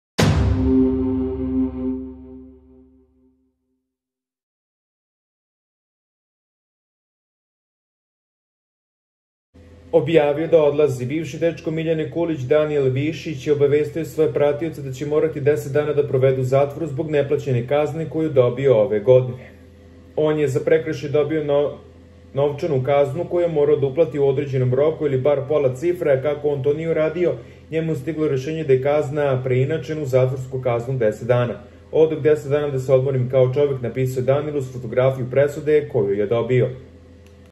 VOLJENI ZADRUGAR NA POTERNICI! REAGOVALA POLICIJA - Mora u ZATVOR! Najavio svima da odlazi
Objavio da odlazi!